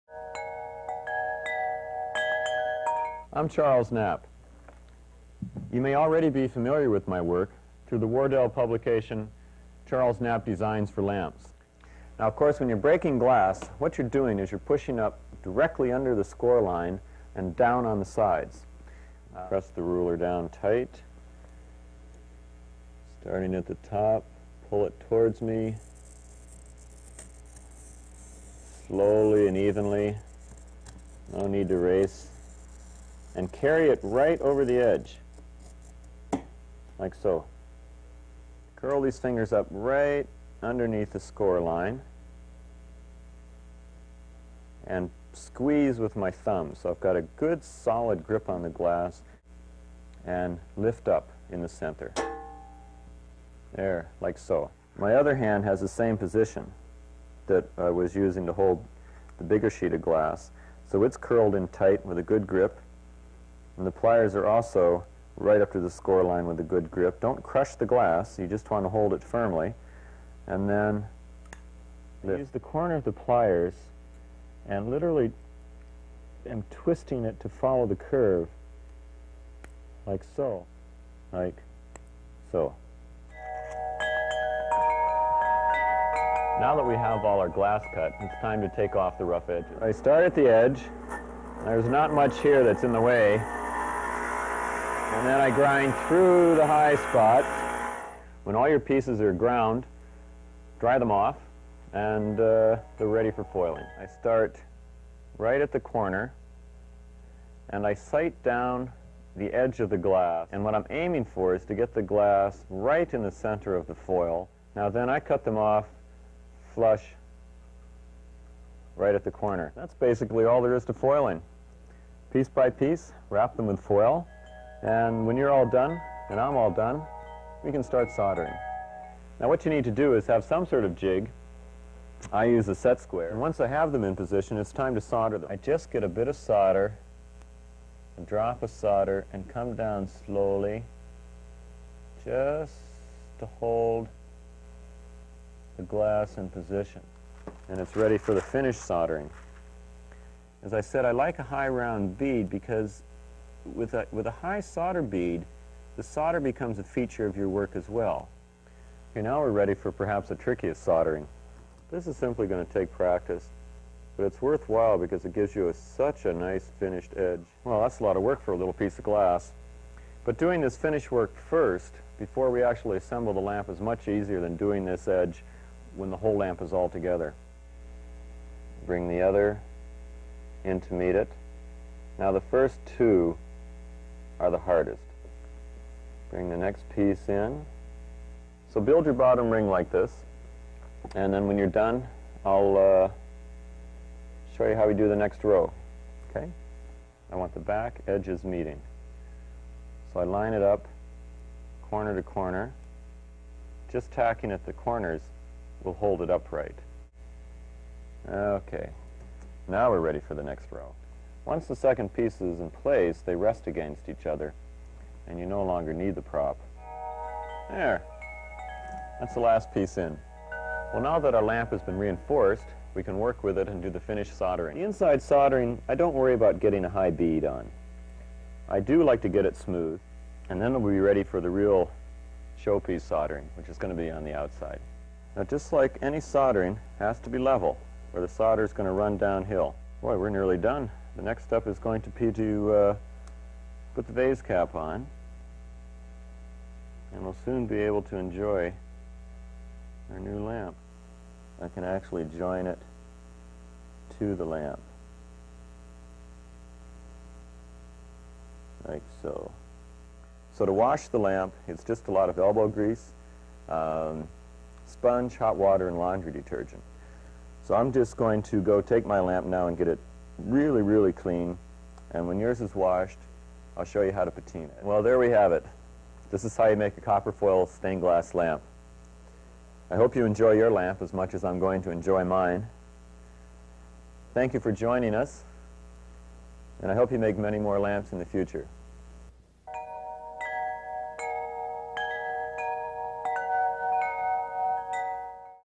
Stained Glass Instructional DVD with Knapp Stained Glass
An excerpt from an instructional DVD created by master-craftsman Charles Knapp, of Knapp Stained Glass.  Charles Knapp is a published stained glass artist with a DVD and two books for DIY stained glass creations.